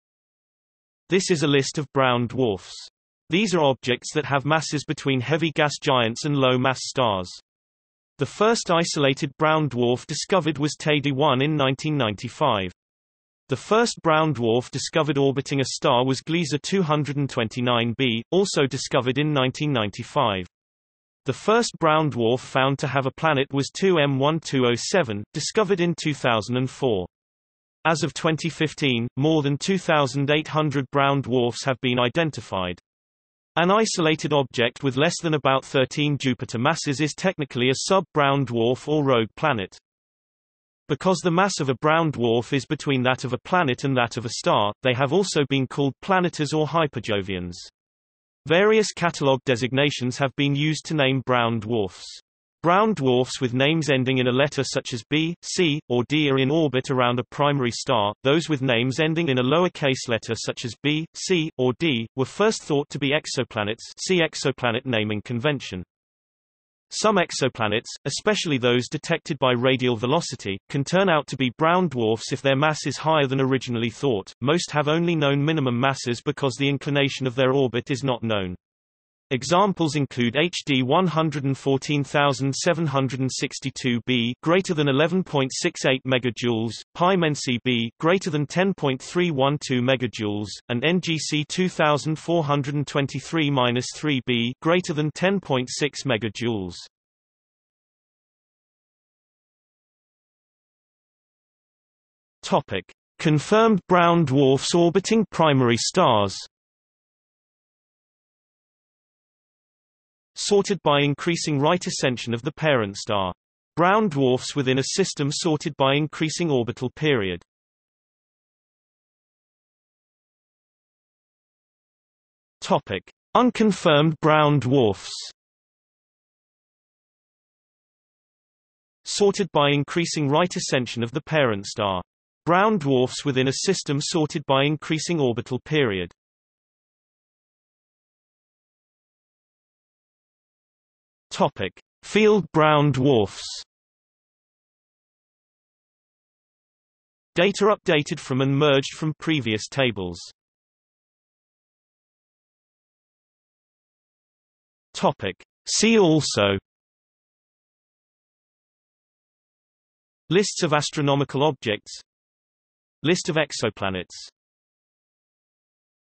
List of brown dwarfs | Wikipedia audio article
00:01:38 1 Confirmed brown dwarfs orbiting primary stars
00:01:59 2 Unconfirmed brown dwarfs
00:02:17 3 Field brown dwarfs
00:02:31 4 See also

- Socrates

SUMMARY
This is a list of brown dwarfs. These are objects that have masses between heavy gas giants and low-mass stars. The first isolated brown dwarf discovered was Teide 1 in 1995. The first brown dwarf discovered orbiting a star was Gliese 229 B, also discovered in 1995. The first brown dwarf found to have a planet was 2M1207, discovered in 2004. As of 2015, more than 2,800 brown dwarfs have been identified. An isolated object with less than about 13 Jupiter masses is technically a sub-brown dwarf or rogue planet.
Because the mass of a brown dwarf is between that of a planet and that of a star, they have also been called planetars or hyperjovians. Various catalog designations have been used to name brown dwarfs. Brown dwarfs with names ending in a letter such as B, C, or D are in orbit around a primary star; those with names ending in a lower-case letter such as b, c, or d, were first thought to be exoplanets (see Exoplanet naming convention).
Some exoplanets, especially those detected by radial velocity, can turn out to be brown dwarfs if their mass is higher than originally thought: most have only known minimum masses because the inclination of their orbit is not known. Examples include HD 114762 b (11.68 MJ), Pi Mensae b (10.312 MJ), and NGC 2423-3 b (10.6 MJ).